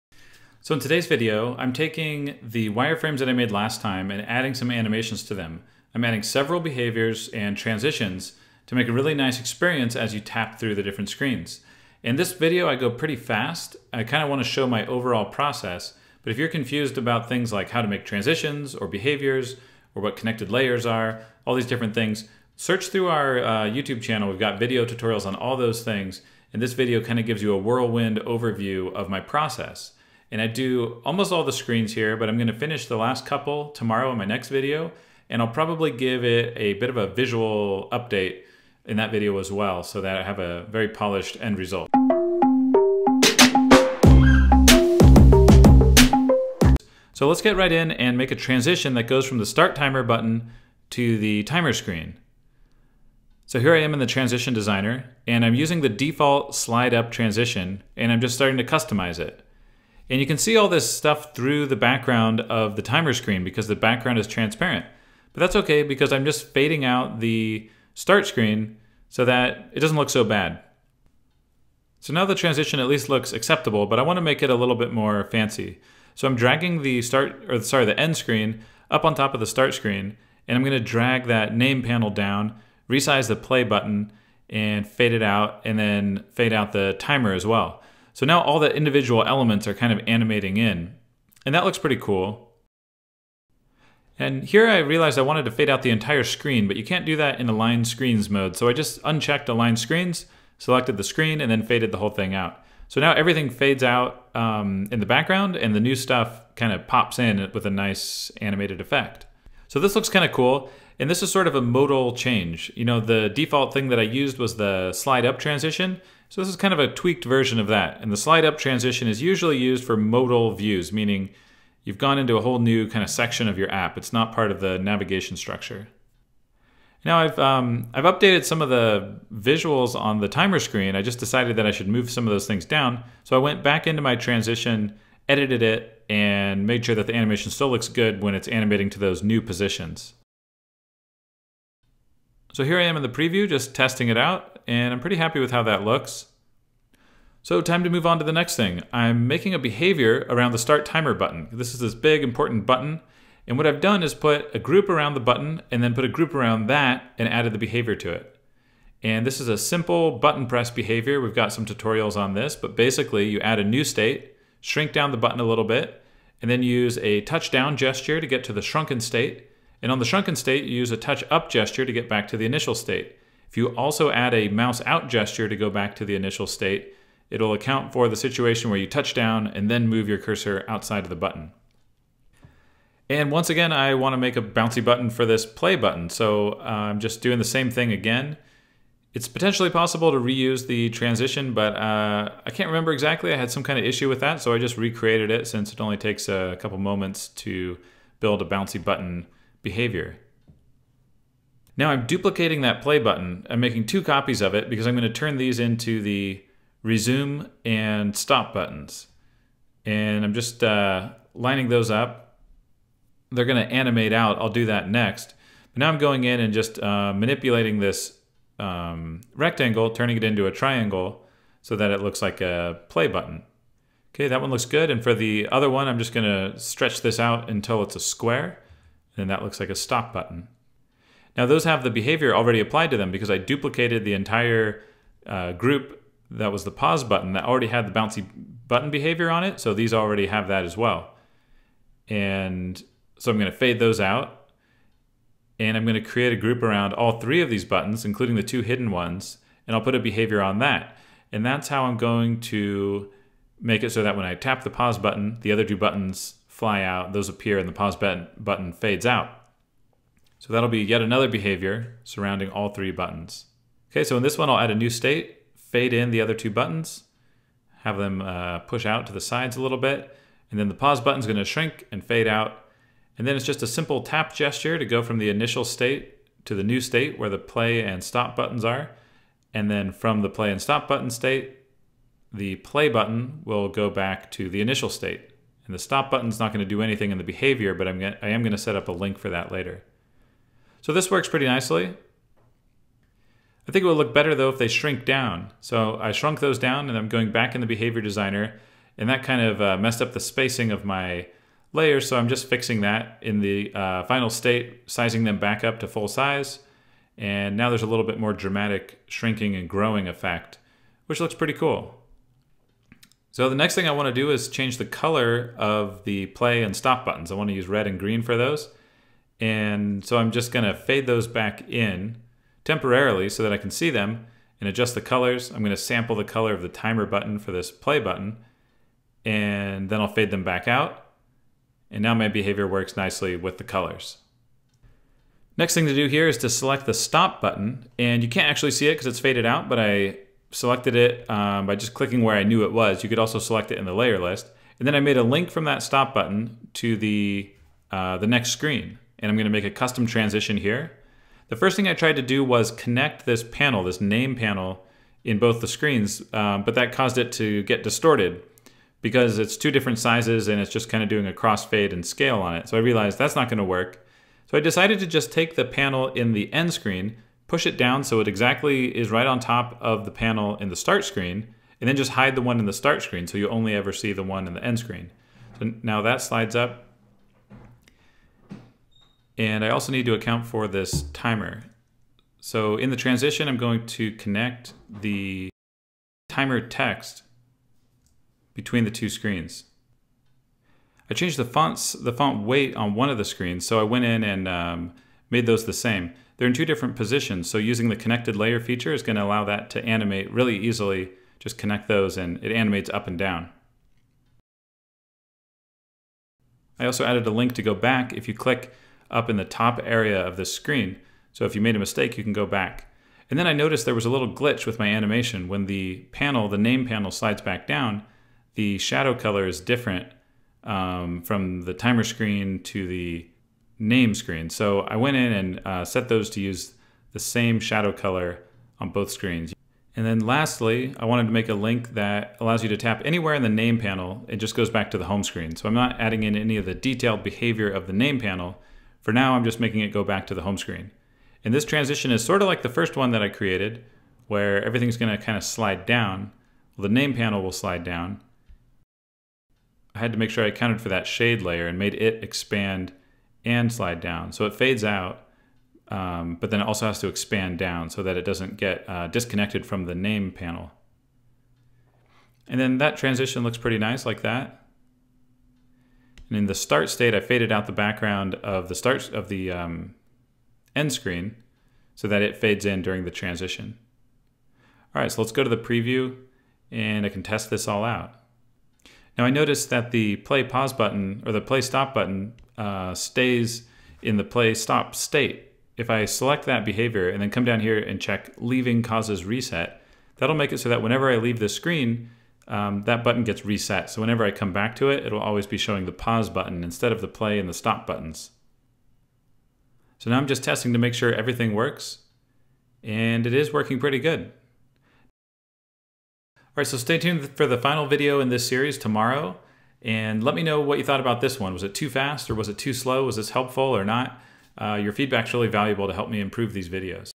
Average Timer Part 4 - adding awesome animations
I took my wireframes from the last video and added lots of cool transitions and behaviors!